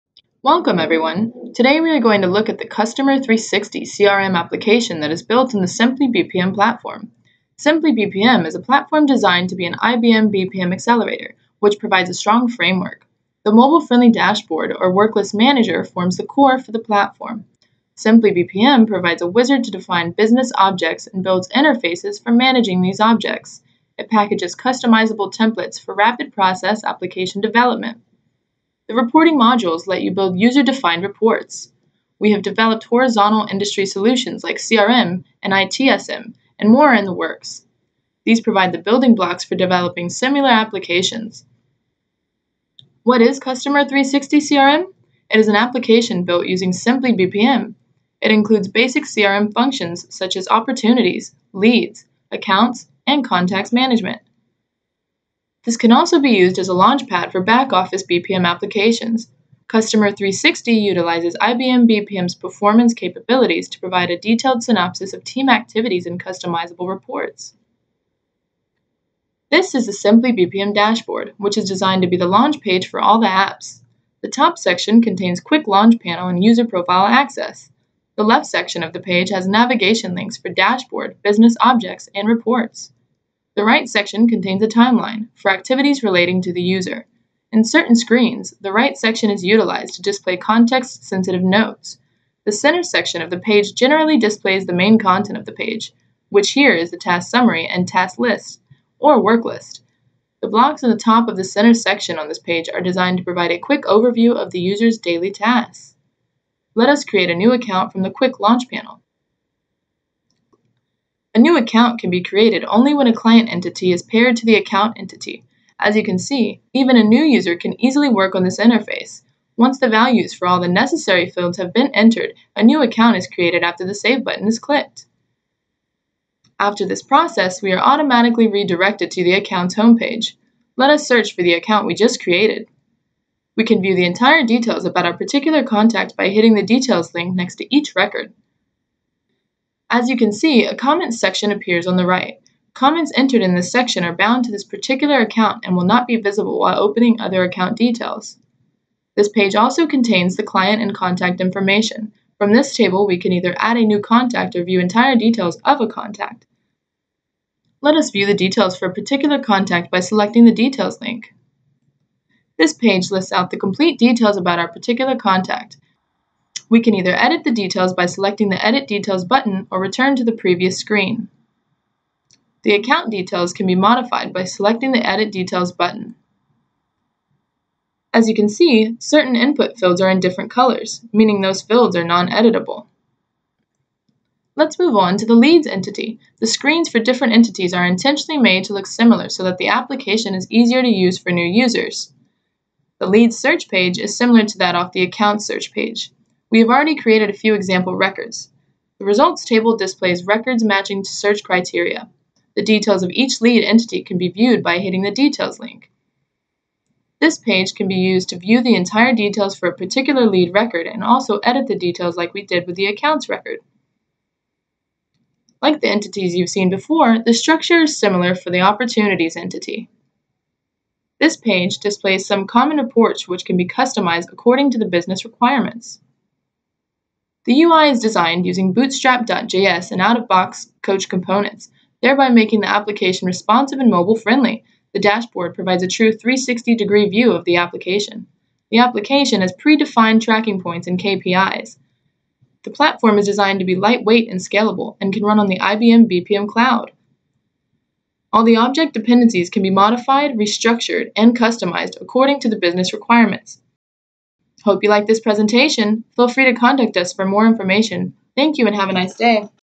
SimplyBPM - Customer 360 Demo
Customer 360 CRM Solution Developed using SimplyBPM to run on IBM BPM